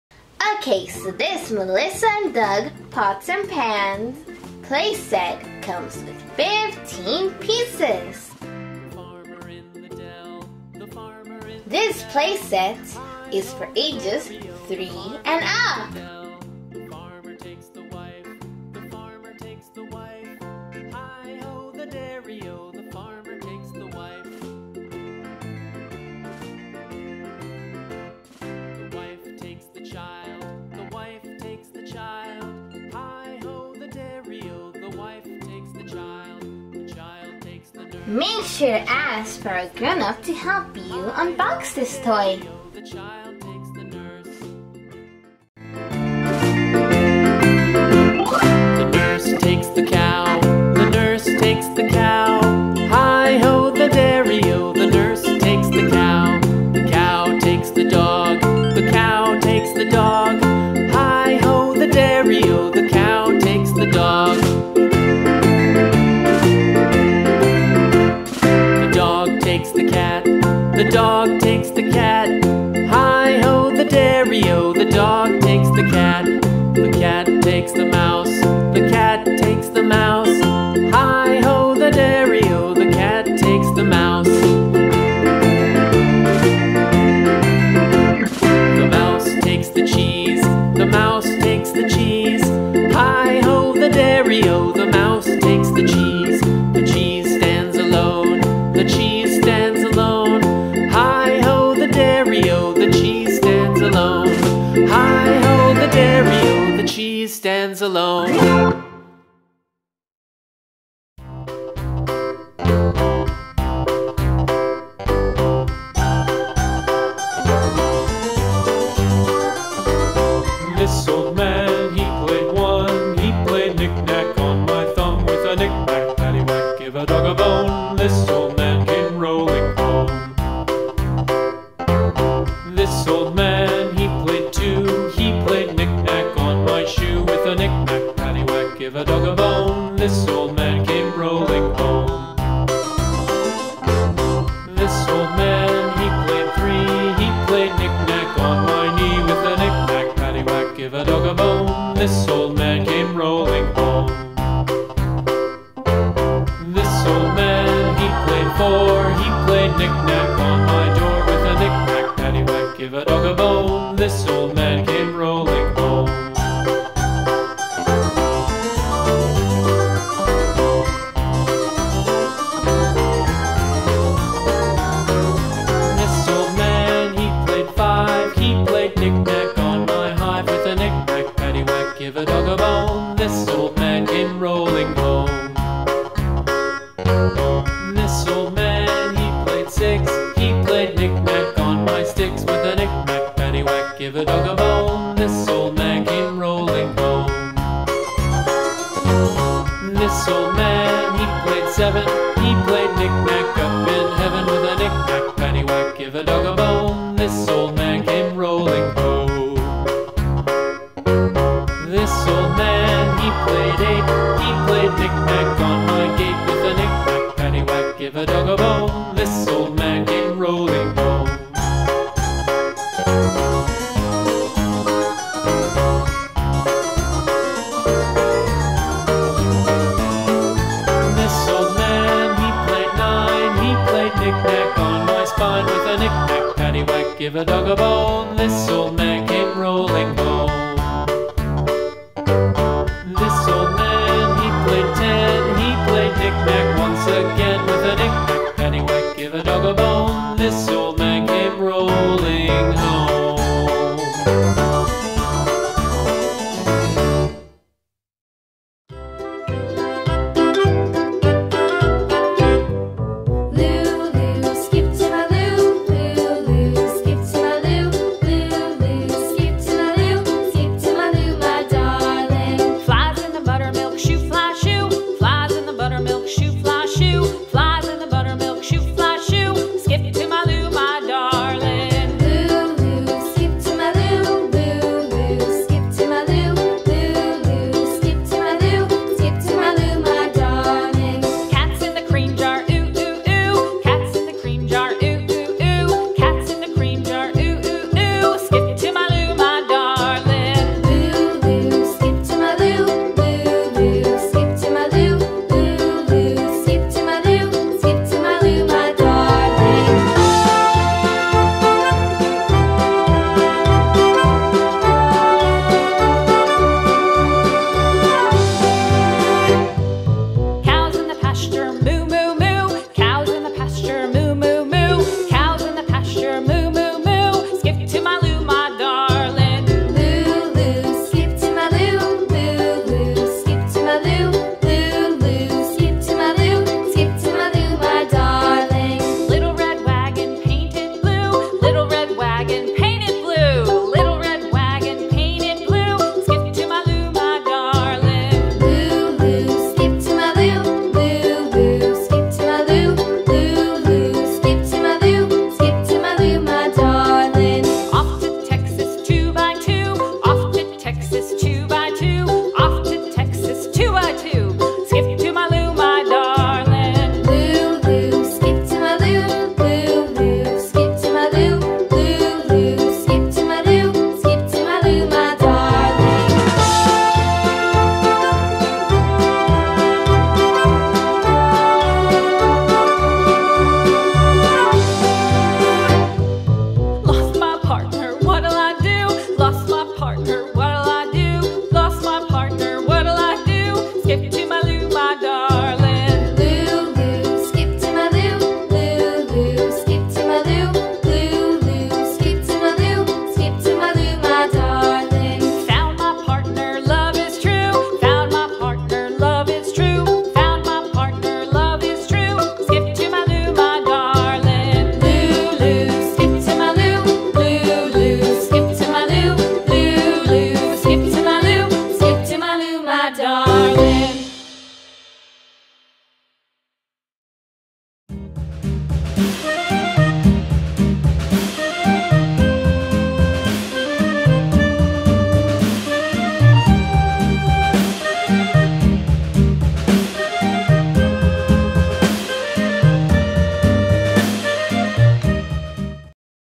Melissa & Doug Let's Play House! Stainless Steel Pots & Pans Play Set
Boil, strain, stir, and saute! This pots, pans, and cooking utensils set is a beautiful way to cook up pretend play! Bright and shiny, the pretend play cookware is generously sized for a more realistic look and feel. For children ages 3 years and older.

HAND POLISHED STAINLESS STEEL:15-Piece polished stainless steel pots, pans, and utensils pretend play cooking set. Includes wire storage rack.

SET INCLUDES: Stainless steel wok, griddle, saute pan, pot, stock pot, colander, insert colander (fits in stock pot), 2 multi-fit lids, spatula, slotted spoon, and storage rack; wooden spoon, spatula, and slotted spatula.

HIGH QUALITY AND SAFE CONSTRUCTION: Handles have sturdy riveted joints that won’t weaken or wear. Stands up to years of play.

GREAT GIFT FOR AGES 3 to 8: The Stainless Steel Pots & Pans Play Set for kids makes a great gift for kids ages 3 and up. Add kitchen accessory set to round out the hands on, screen free, pretend play cooking experience.